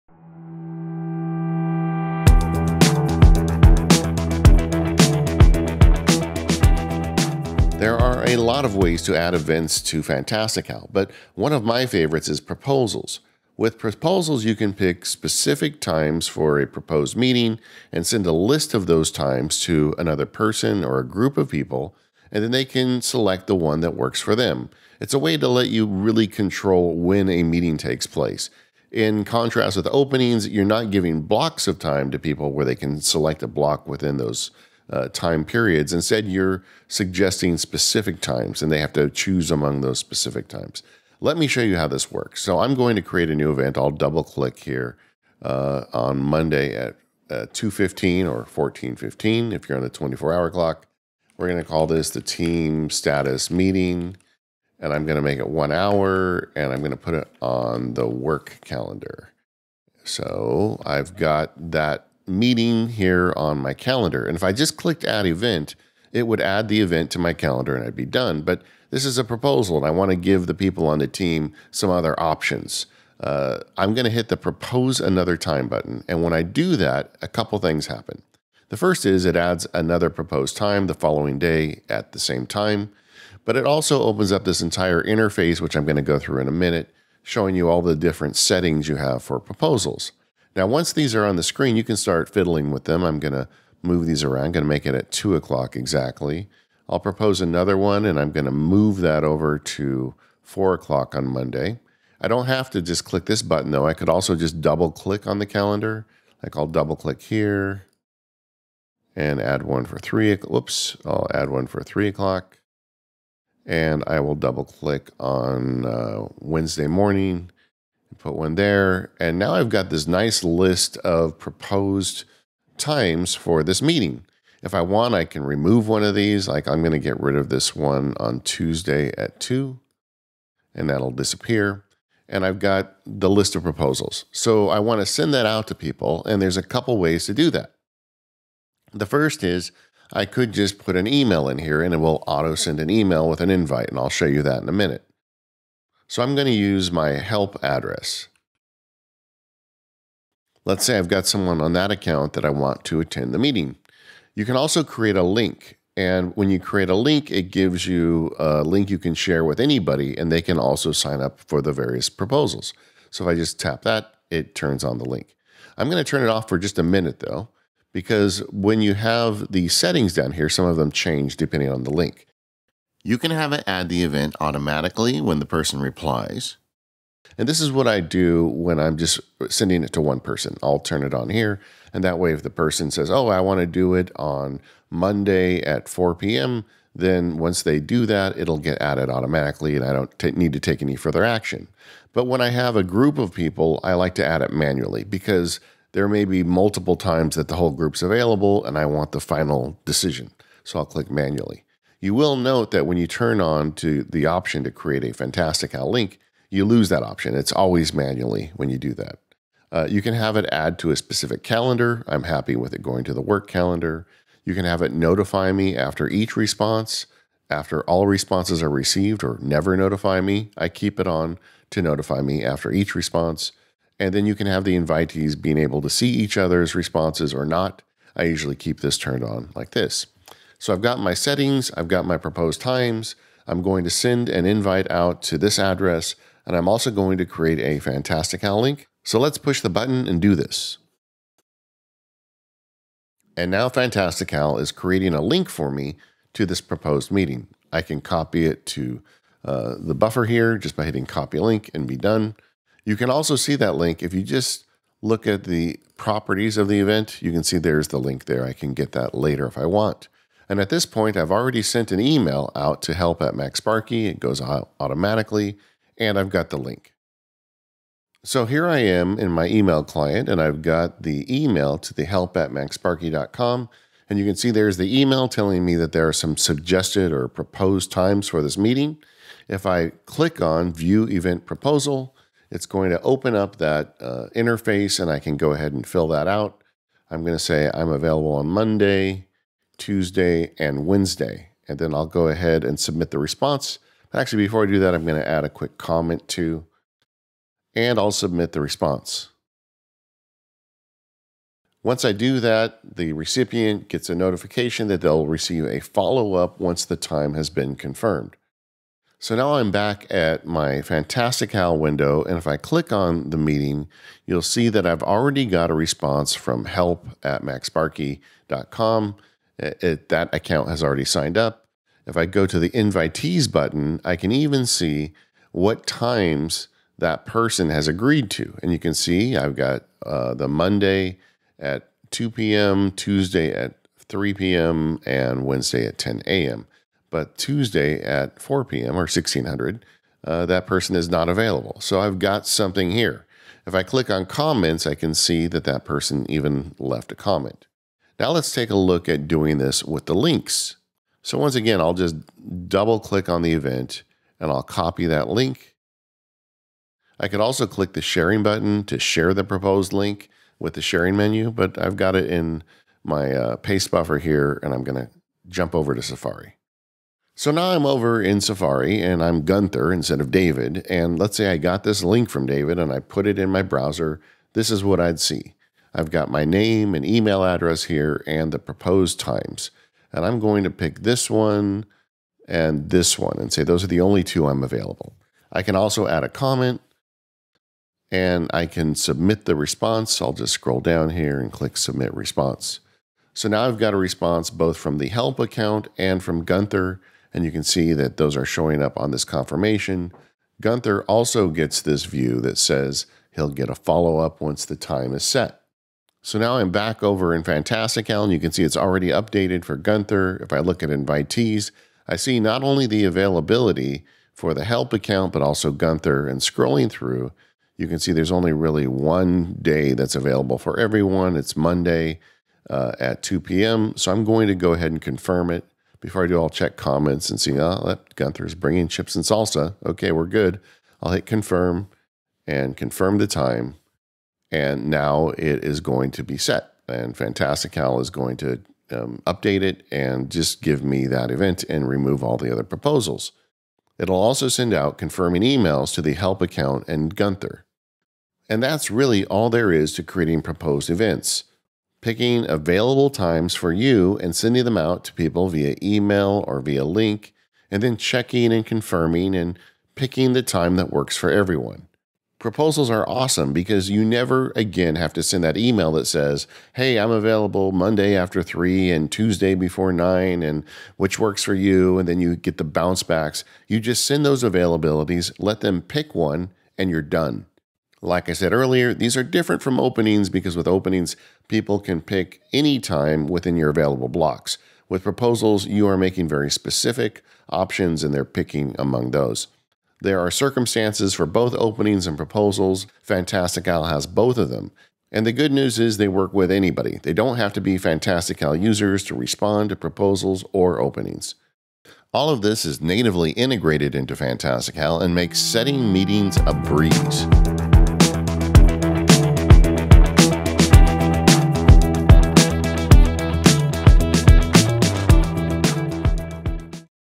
Fantastical - Scheduling with Proposals
Try it for free on Mac, iPad, iPhone, and Apple Watch

Learn how Fantastical Proposals makes it easy for multiple people to vote on the best time to have a meeting.